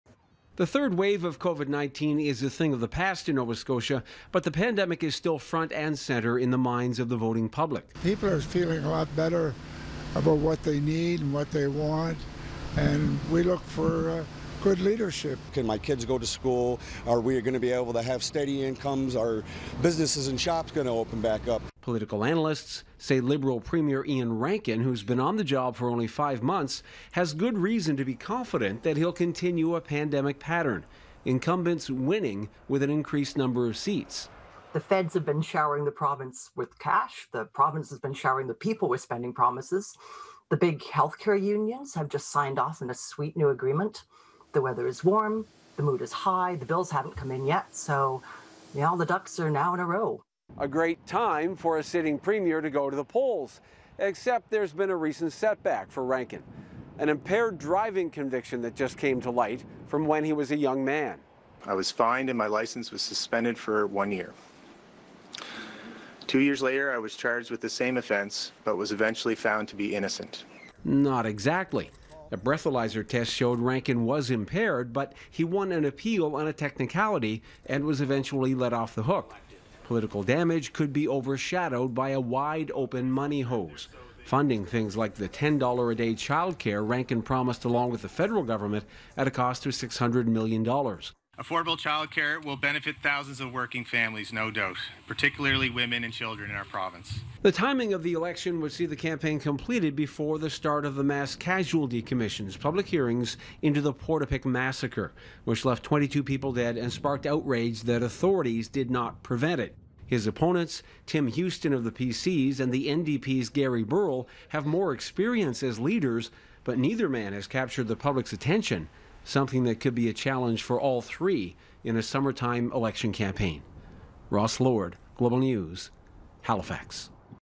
What the timing of Nova Scotia's election means for the campaign
Nova Scotians are heading to the polls on August 17 after Premier Iain Rankin dissolved the legislature to kick off the province's 41st election campaign and the first summer vote since 2003.

Health care and the response to COVID-19 is likely to be at the forefront of voters' minds and experts say it could be the right time for an incumbent premier and new leader of a Liberal party that has won two straight majorities.

As Ross Lord reports, an impaired driving conviction from when he was a young man could create political damage, but recent funding announcements, including $10-a-day childcare, may create enough of a buffer to lead Rankin to his first electoral victory in leadership.